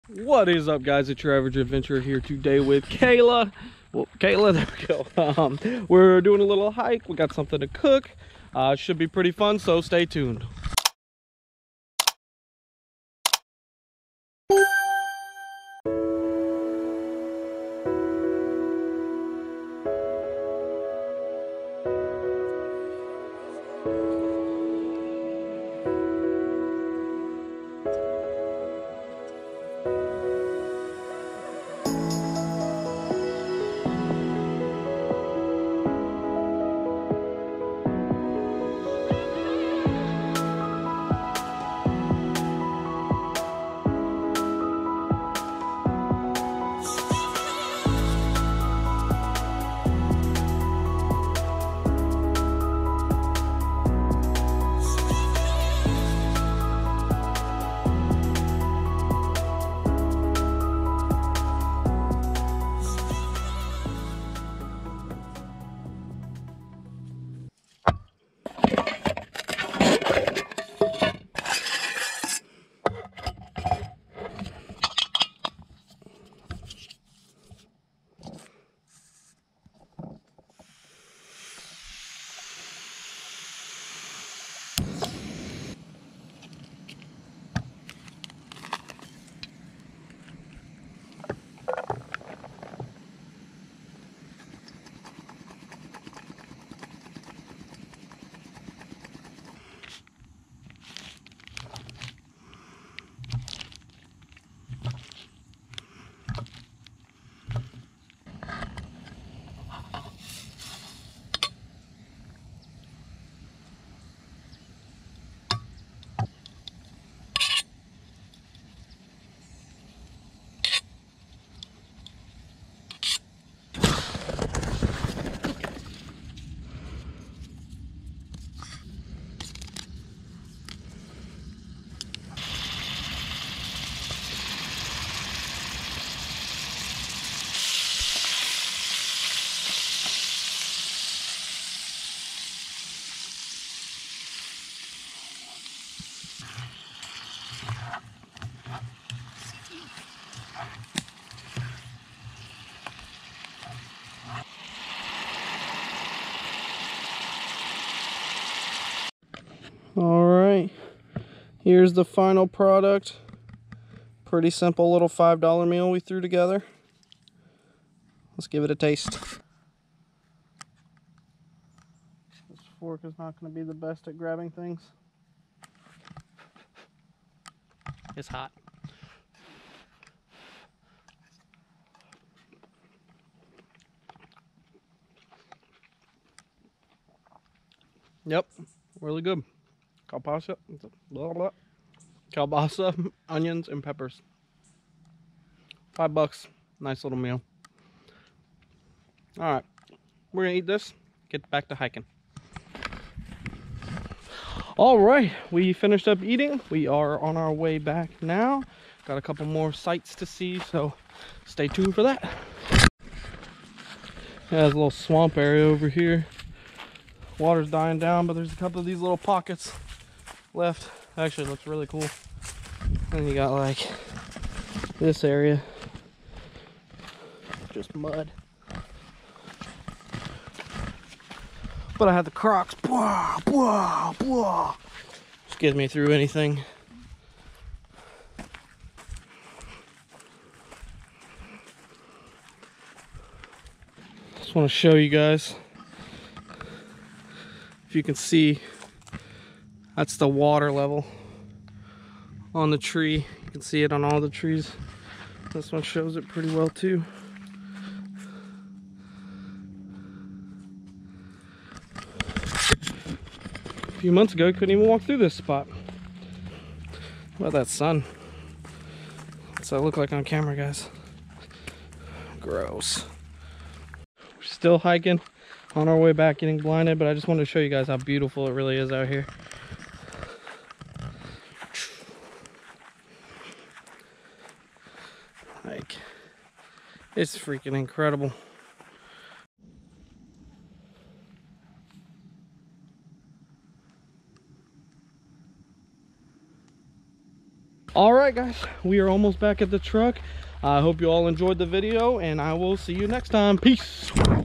Hike N Eat: Sausage, Onions and Peppers On The Trail
It was a beautiful day here in Florida and we wanted to take full advantage! We hiked out, whipped up some dinner and enjoyed being surrounded by nature.